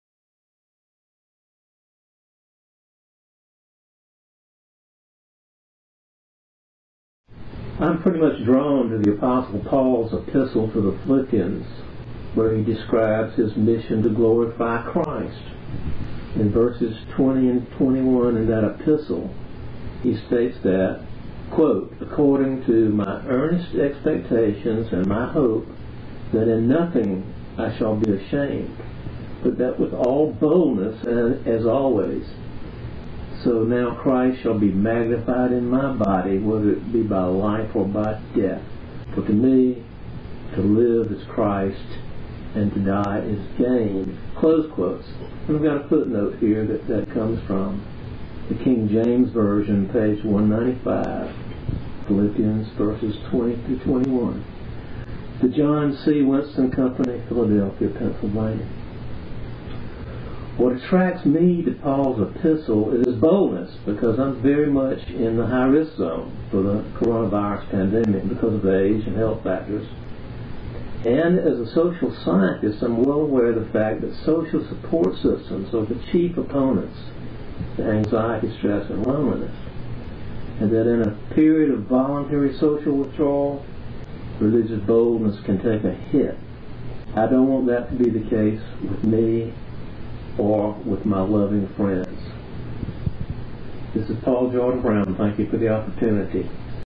Devotional Paul Brown
WSUMC devotional message; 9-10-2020 with member, Paul Brown.